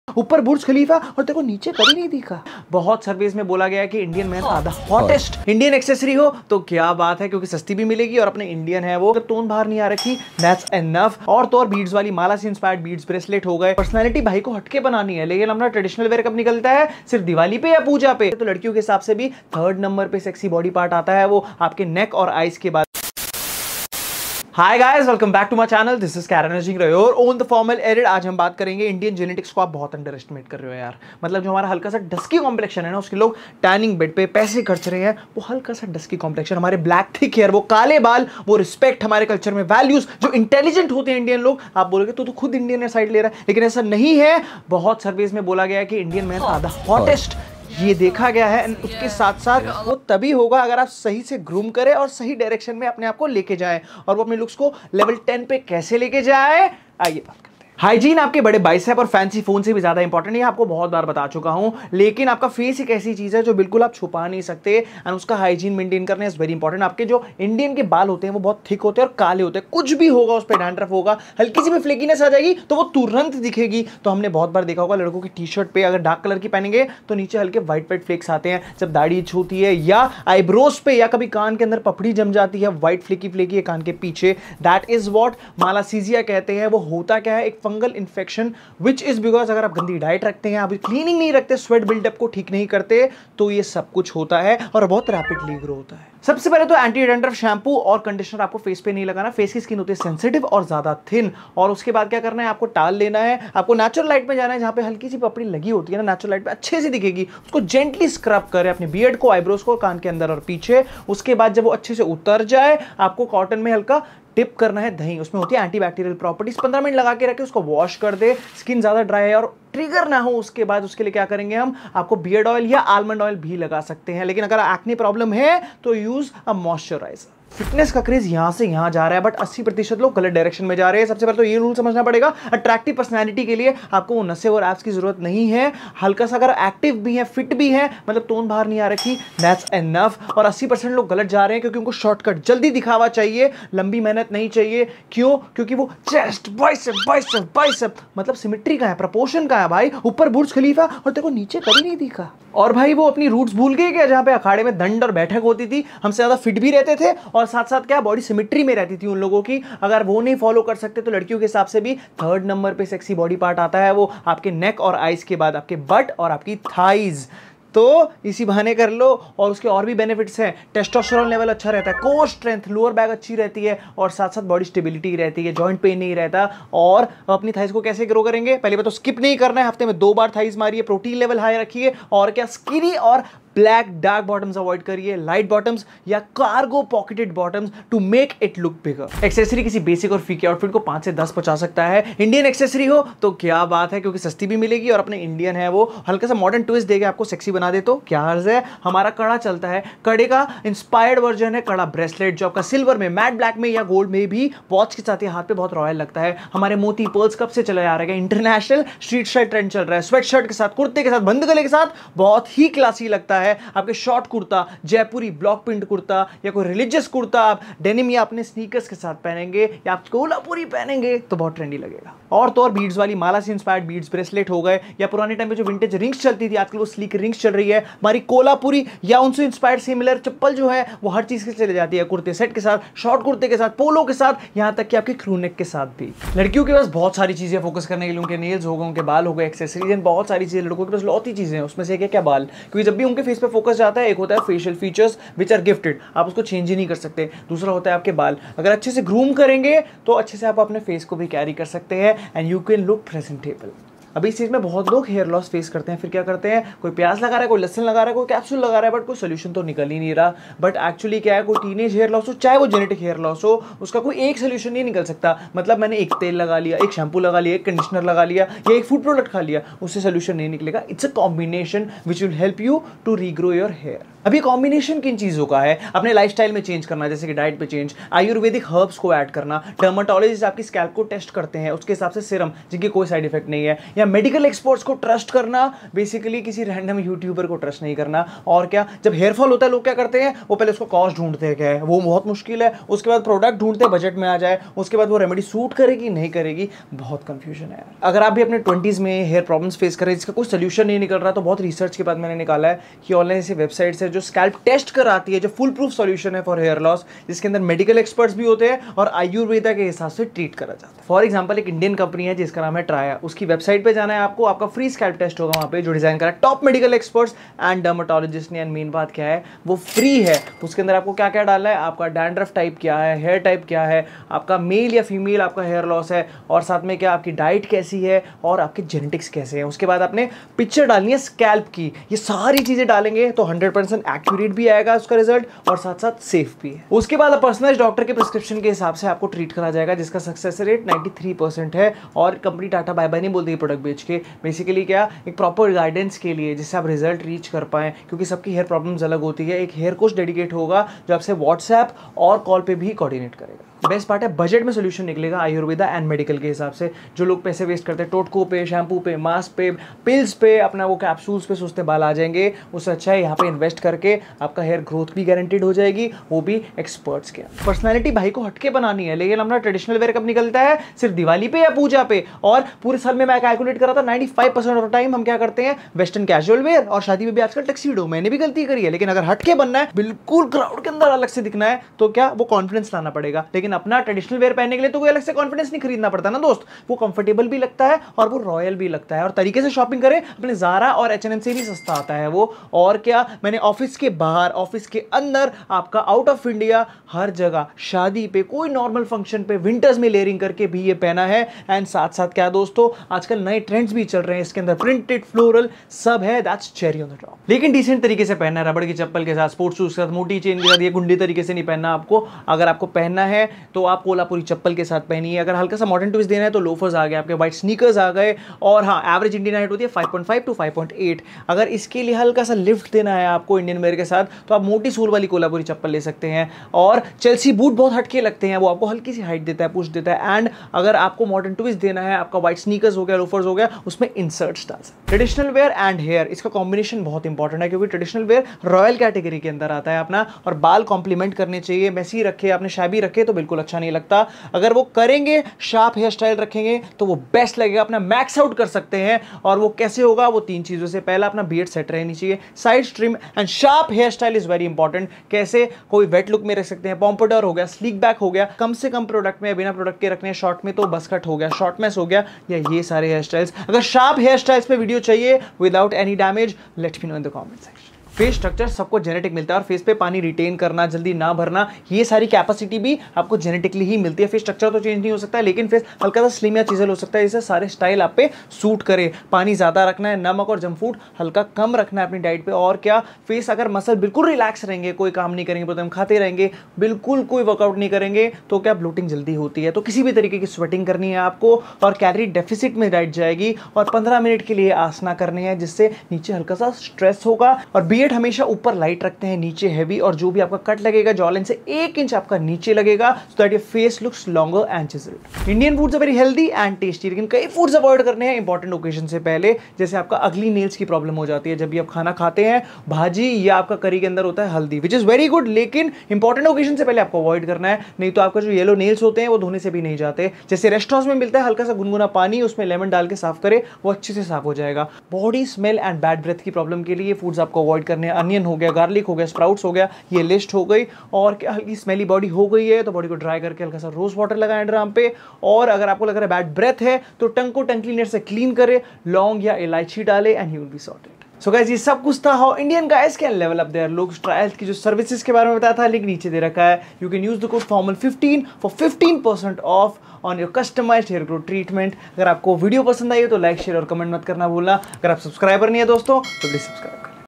Hacks for Indian Boys to look ATTRACTIVE PRO MAX| Desi| Look Handsome | Hairstyles | Lip care| Hindi
Hey Guys! I hope you are doing great.

So in this video
00:00 Trailer
00:26 Why Indian Men Are hot?
00:57 Face Hygiene
02:07 Proportion of your Body
03:09 Indian Accessories
03:56 Indian Teenage Hair loss
06:07 Traditional Wear Everywhere
07:15 Top Hairstyles for Indian Men
07:46 Change Face Shape
08:25 Indian Beauty Foods & Hygiene

Karron S Dhinggra